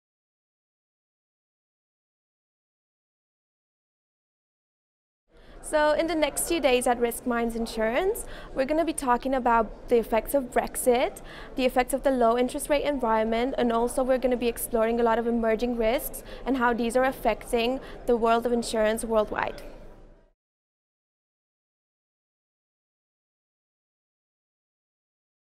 What can we expect at RiskMinds Insurance this week? Producer gives insight
At RiskMinds Insurance 2017, Editor-in-Chief Ana Luiza Olanescu tells us what we can expect from the thought leaders of the risk insurance industry.

Follow the conversation on Twitter @RiskMinds with RMInsurance and LinkedIn.

for the latest in Risk thought leadership.